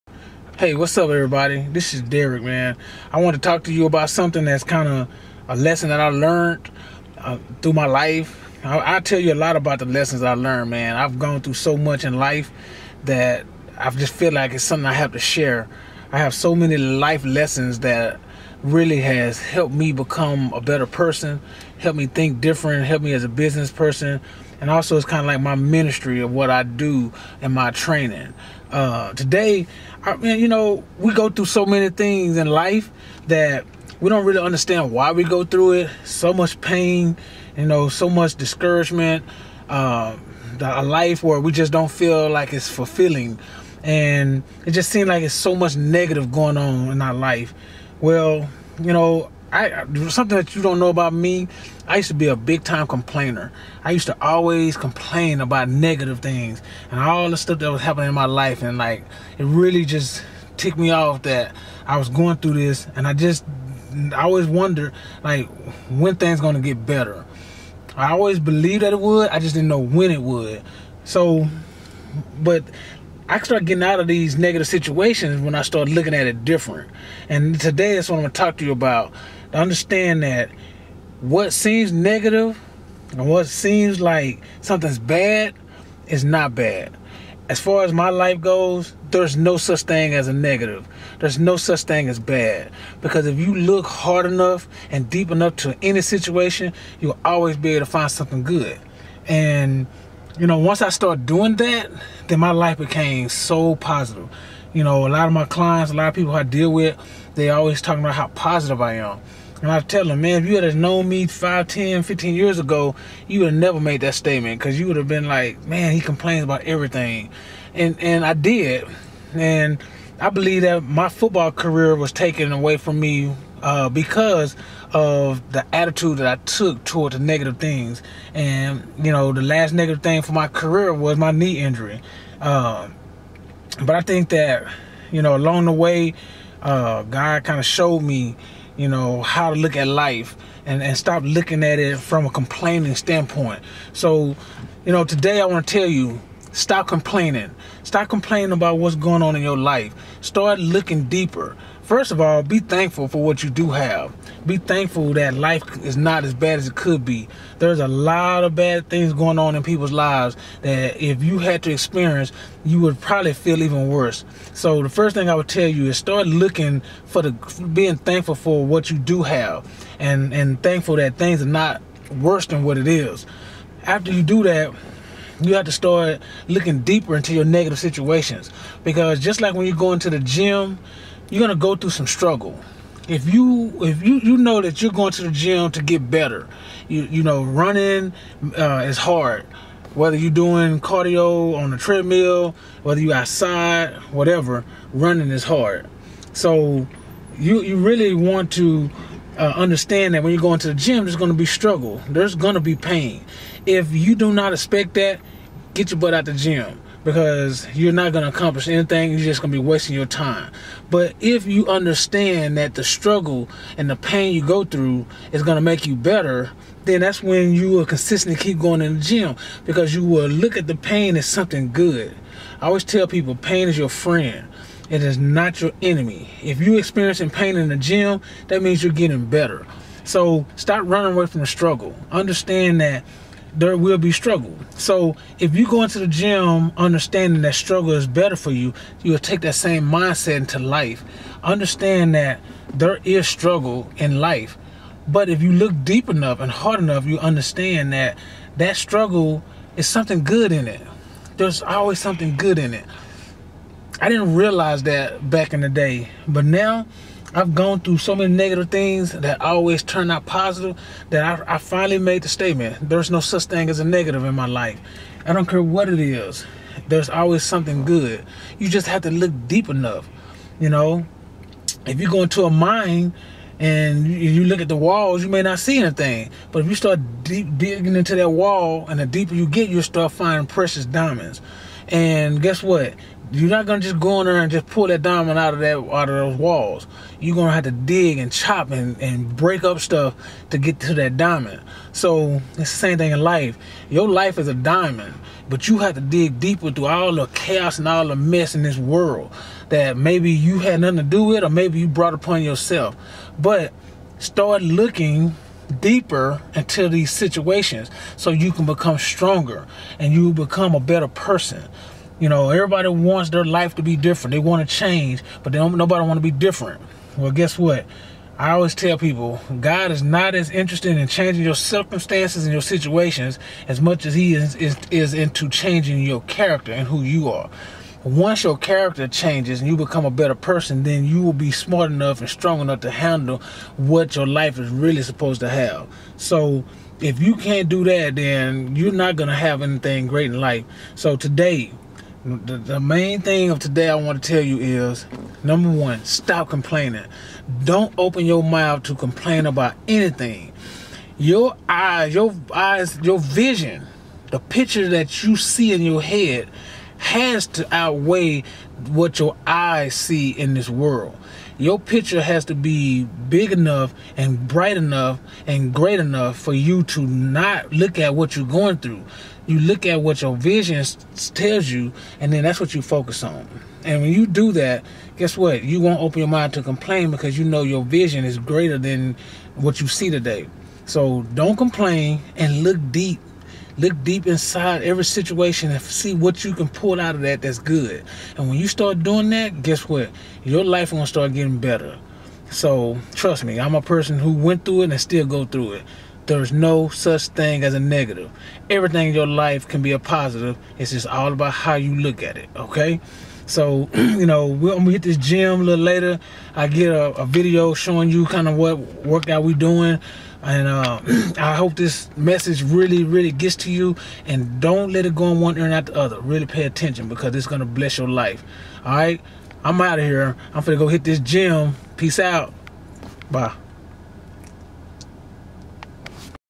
How To Turn A Negative Situation Into A Positive One- Derrick's Mental Diamonds
Don't Let Negativity Stop Your Progressive To A Better Life.

I give you Derrick's Mental Diamonds to help you  through the daily struggles through life. Use these mental diamonds as encouragement to help you strive to your success.